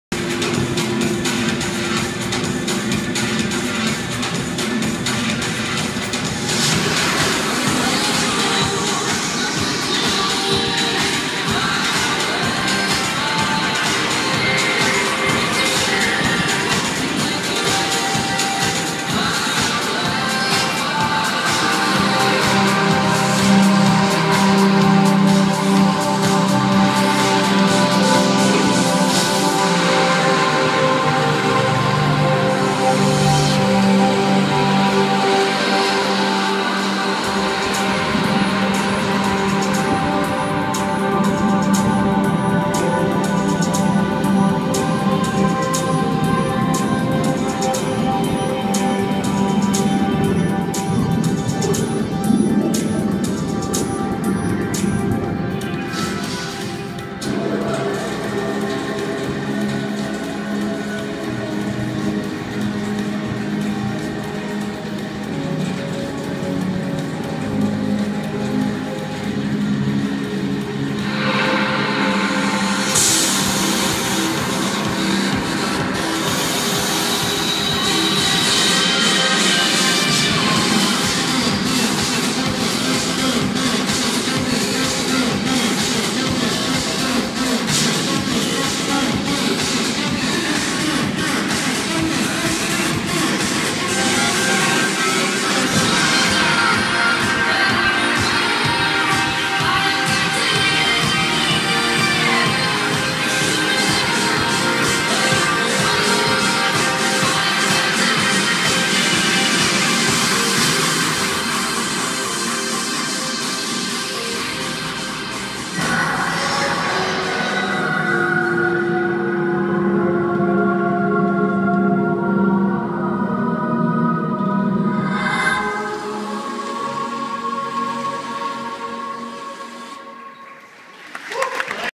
Sydney Town Hall organ in lights
Sydney town hall organ light show at the end of Sydney City Talks featuring Sir Tim Berners-Lee.